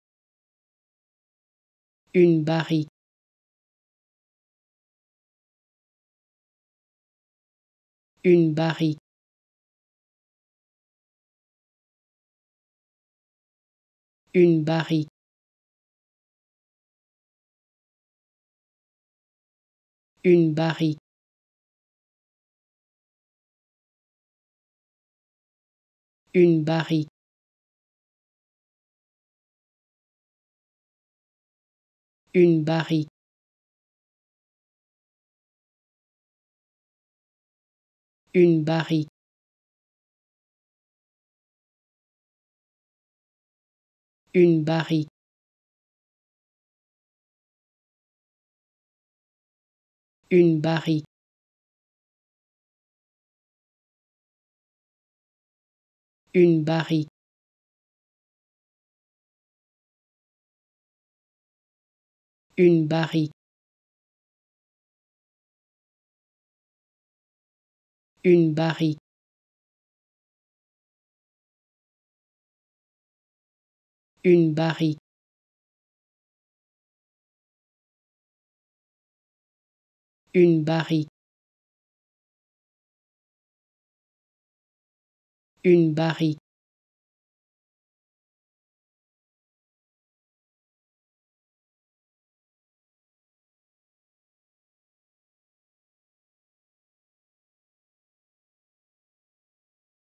How to Pronounce ''Une barrique'' (A barrel) Correctly in French
Learn how to say and properly pronounce ''Une barrique'' in French with this free pronunciation tutorial.